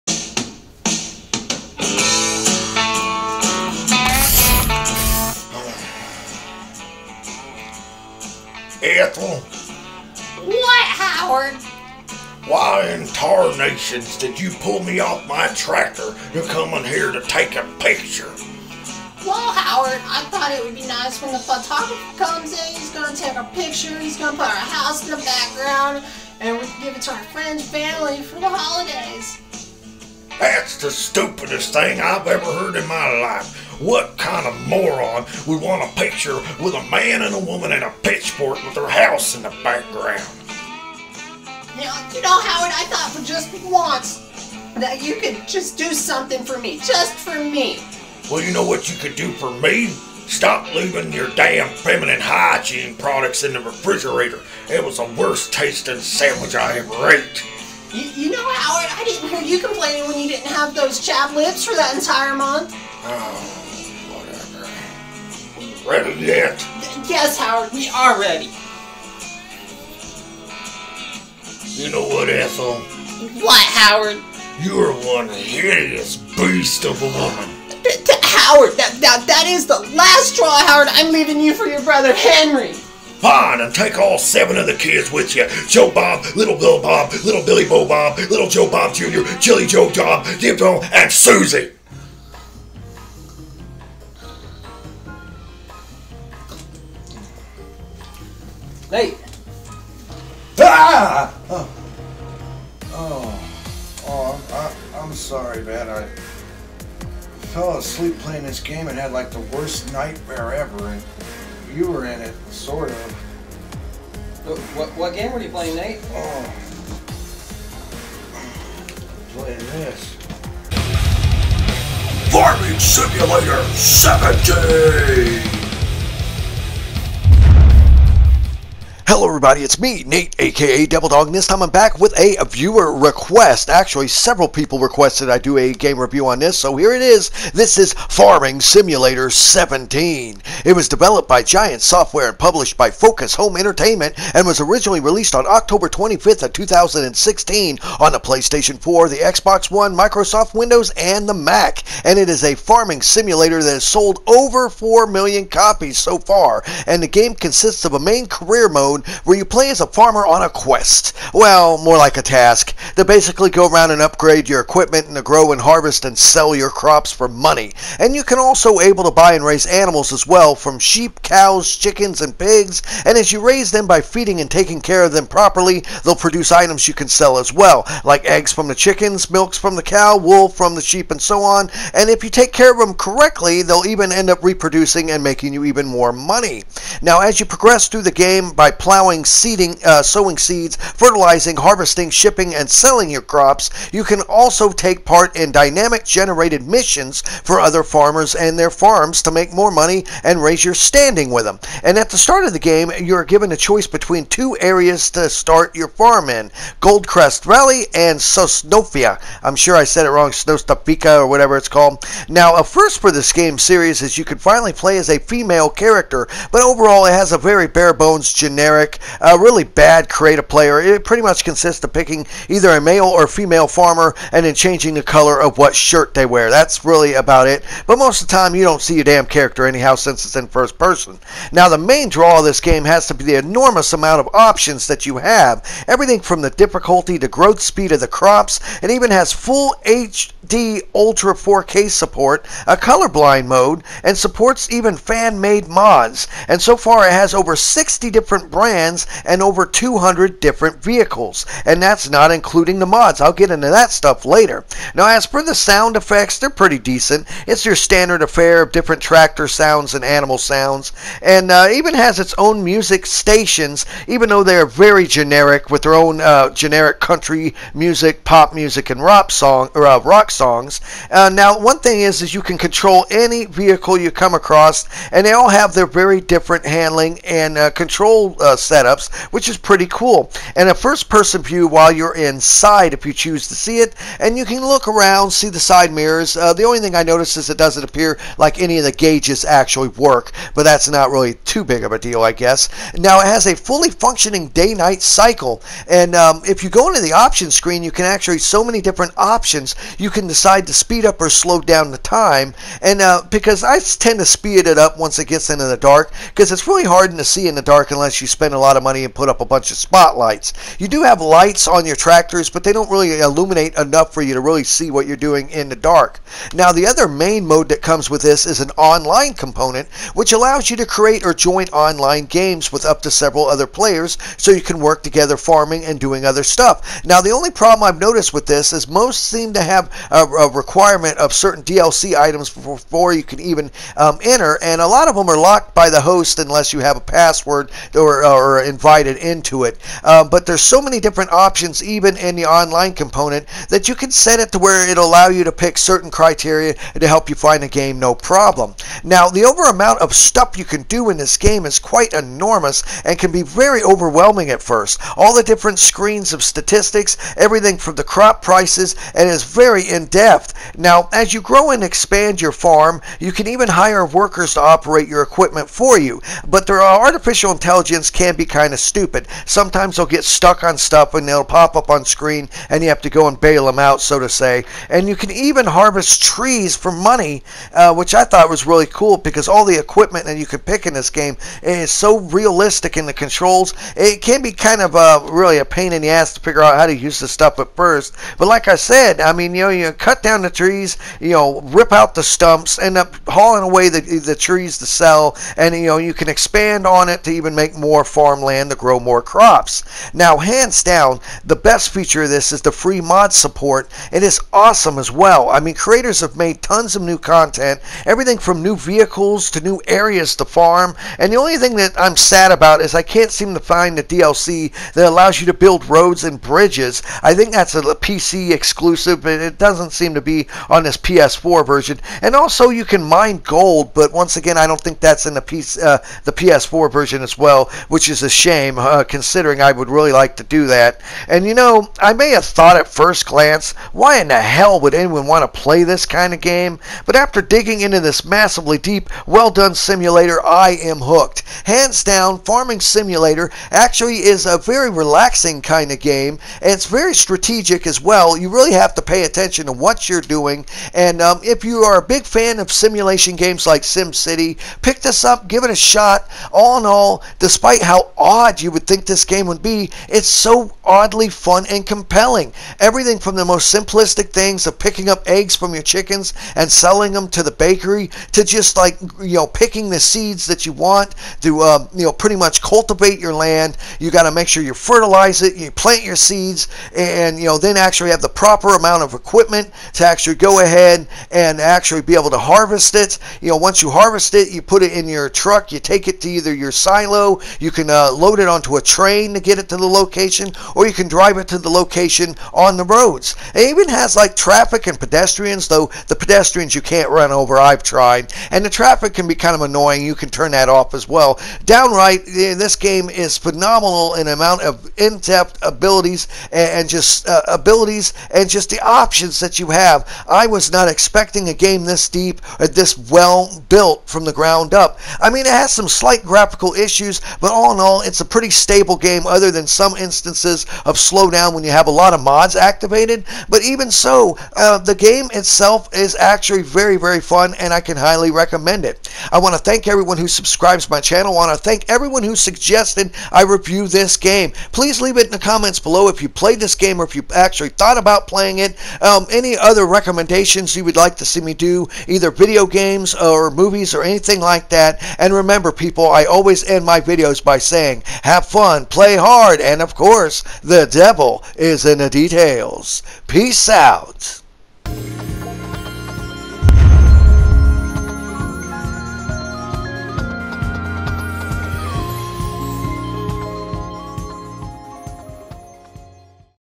Farming Simulator 17 Epic Review
my epic review of the ps4 version of Farming Simulator 17, a really in-depth farming simulator packed full of details and options